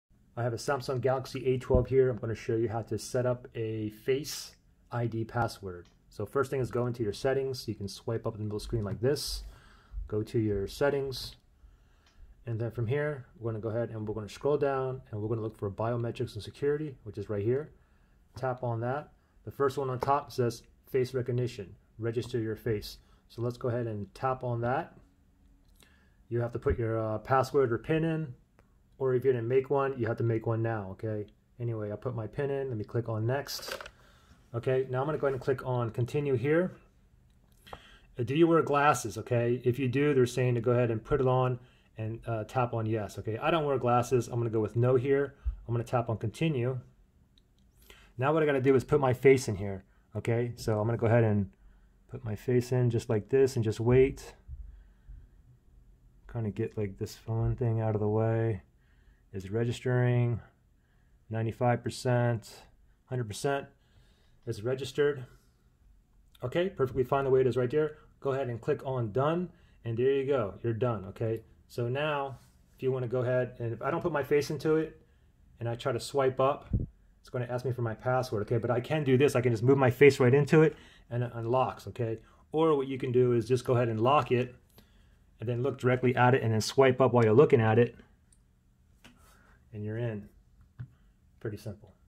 Galaxy A12: How to Setup / Add Face ID Password
I show you how to setup / add Face recognition ID password on the Samsung Galaxy A12. Hope this helps.